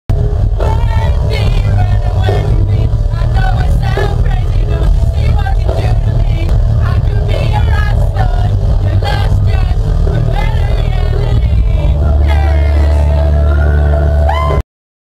All Time Low - What do you like about All Time Low?
O2 asked fans at O2 Academy Birmingham: "What do you like about All Time Low?"